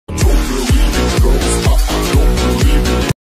part 5 Ghost map (finished version)
this part is for squid pastas Ghost map, its very small but took a few hours- anyway, I'm excited to see what the map looks like when its finished, I've joined many maps but none have been finished so I really just wanna see a map that I joined actually being finished you know? its just cool to see everyone's else's parts and then you would see yours